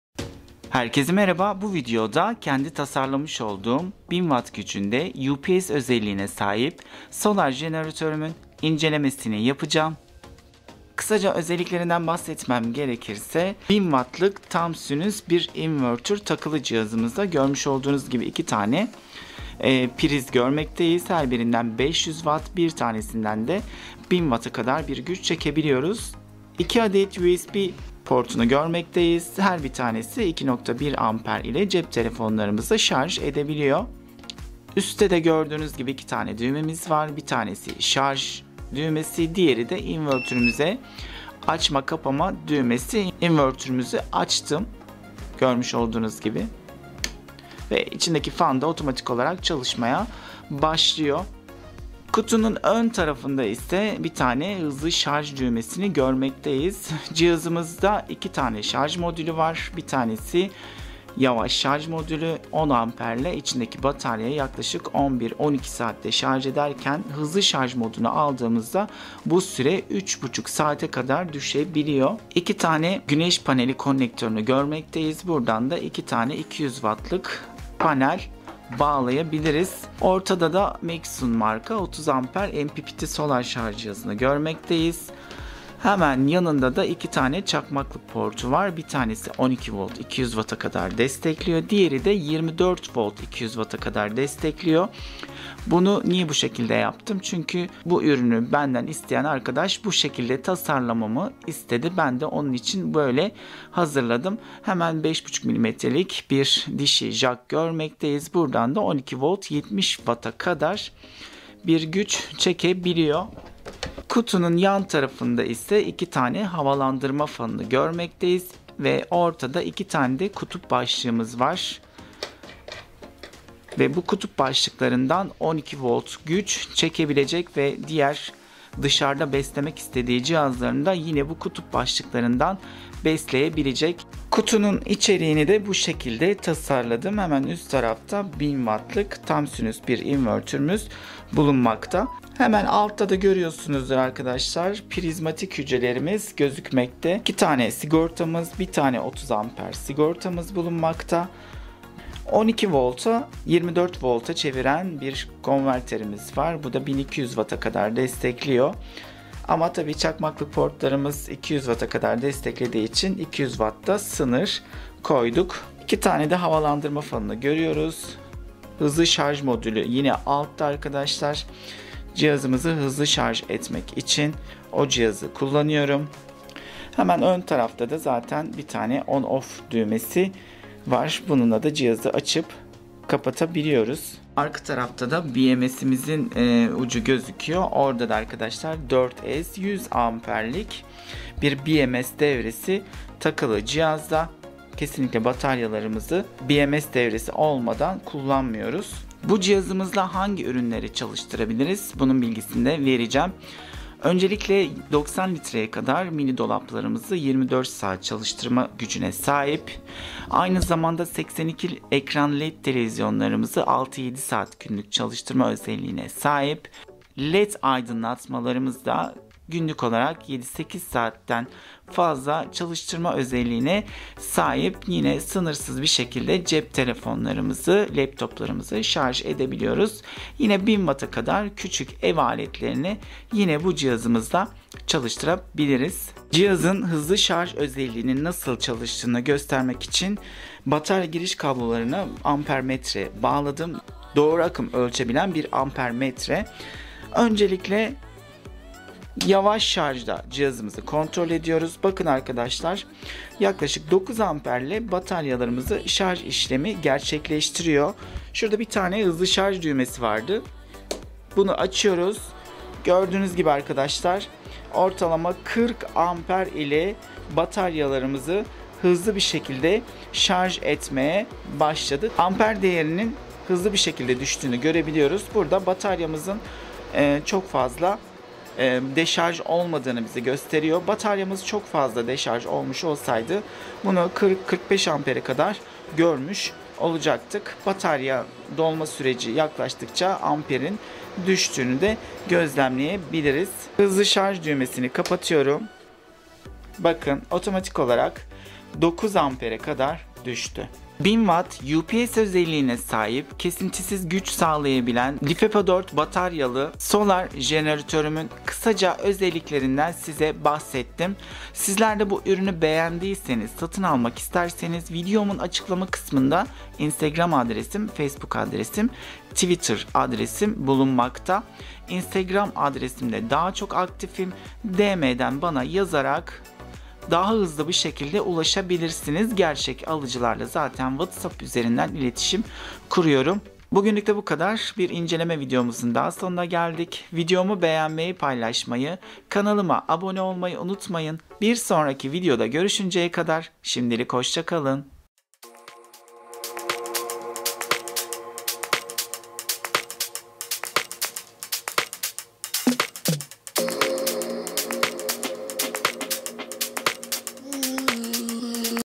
1000w ups güçkaynagı El yapımı BEDAVA ELEKTİRİK SOLAR ENERJİ
1KW KESİNTİSİZ GÜÇ KAYNAGI 12v 120A LİFEPO4 BATARYALI  TAM SİNÜS AKILLI İNVERTERLİ 30a mpptli SOLAR JENARATÖR